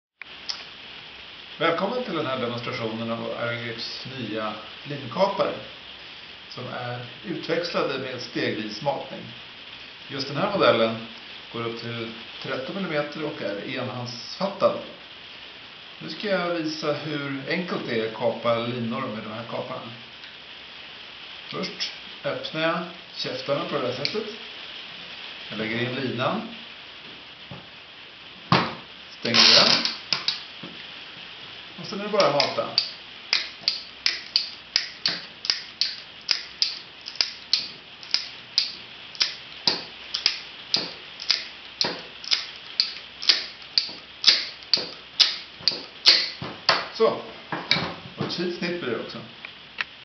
Wiresax SCS13
Så här går det till att kapa stållinor med IronGrips nya wiresax SCS13